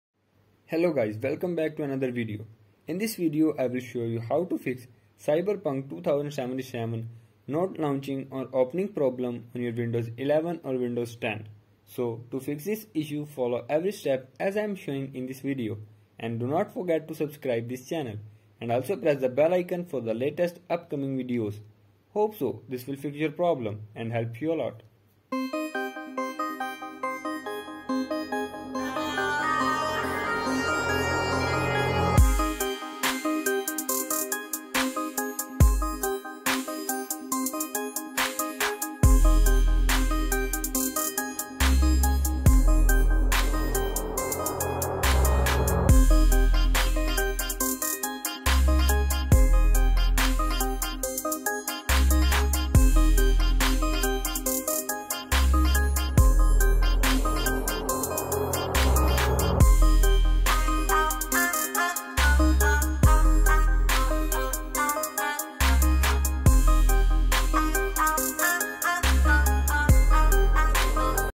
How to Fix Cyberpunk 2077 Not Launching Windows 10 / 11 || 2023 Fix
0:18  Cyberpunk 2077 Not Launching problem fixed